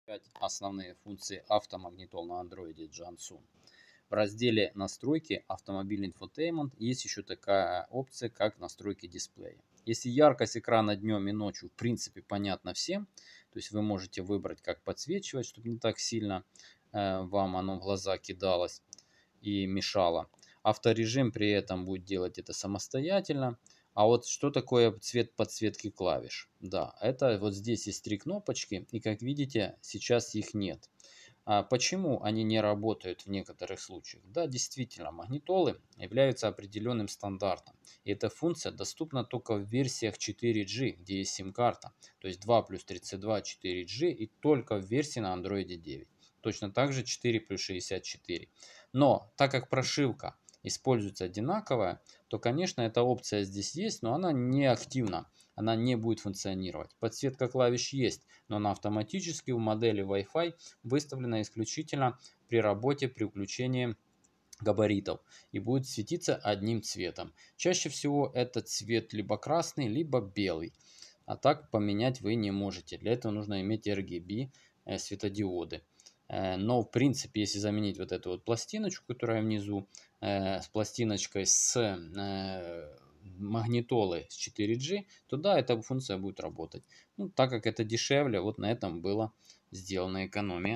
Setting the backlight of control buttons and why they do not work in Junsun android radio
Настройка подсветки кнопок управления и почему они не работают в Junsun андроид магнитолах

Подробности : sk.ks.ua